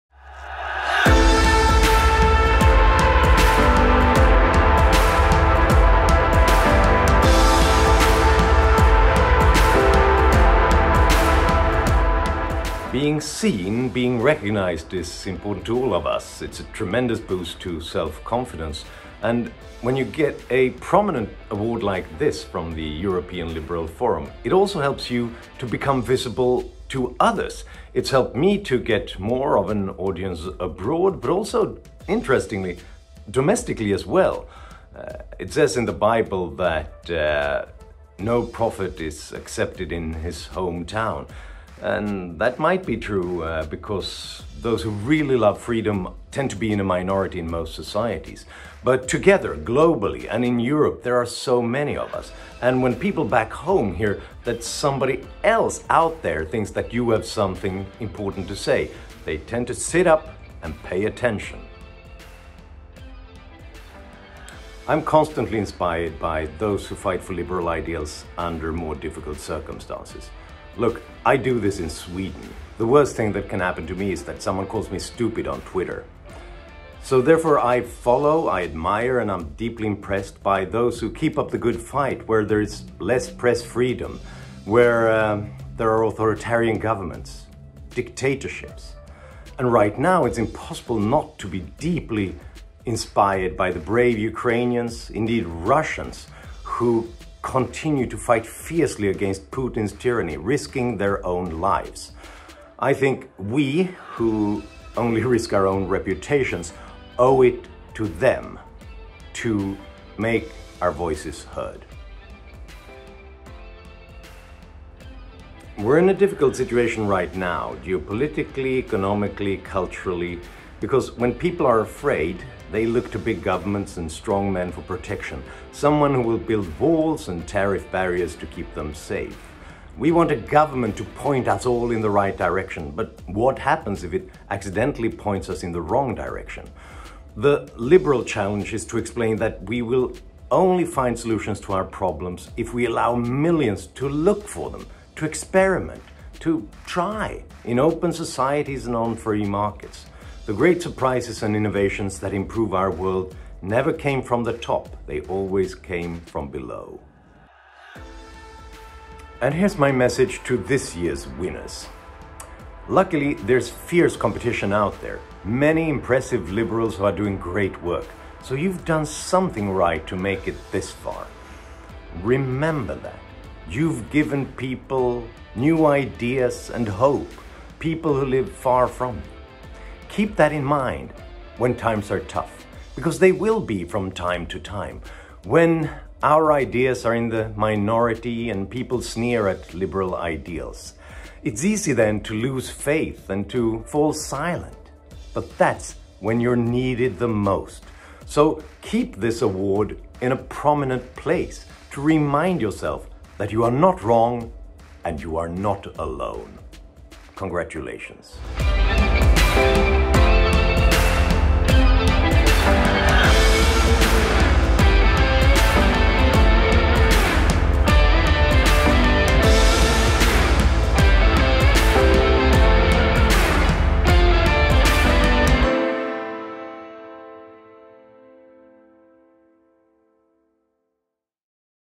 Liberal Award Winner 2021: A discussion with Political Disruptor Johan Norberg
Johan Norberg is the ELF Liberal Awards 2021 winner for the Political Disruptor category.

More information on this year's ceremony in Dublin